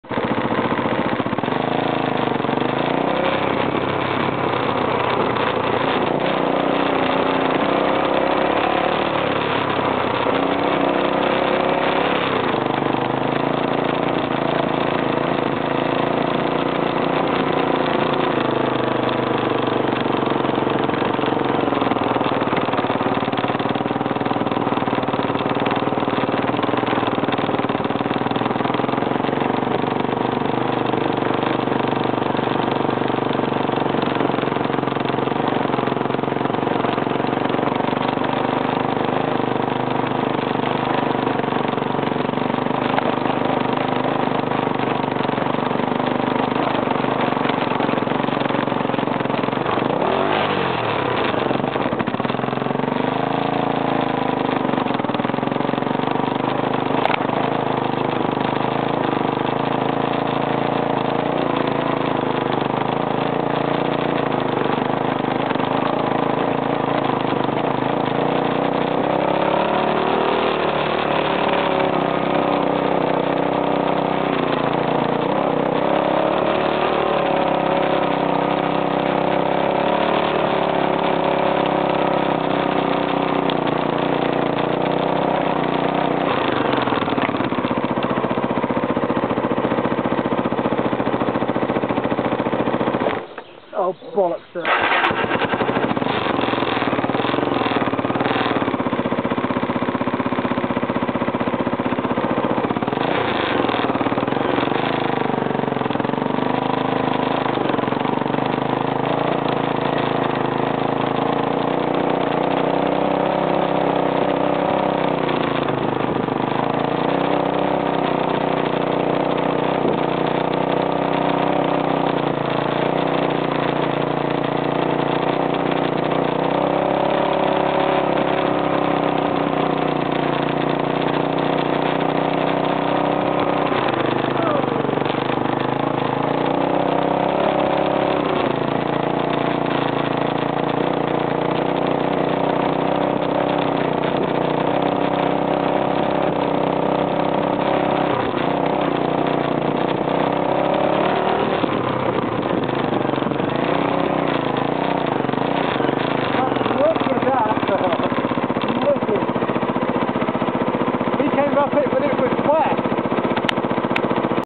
Husaberg Devon Green Lanes Bideford Area North Devon
Devon Green Lanes a mix of wonderful green lanes in the torrington and bideford area including Buckland Brewer and Monkleigh Husaberg 450 enduro XR250 POR 250 day organised by Steve Peck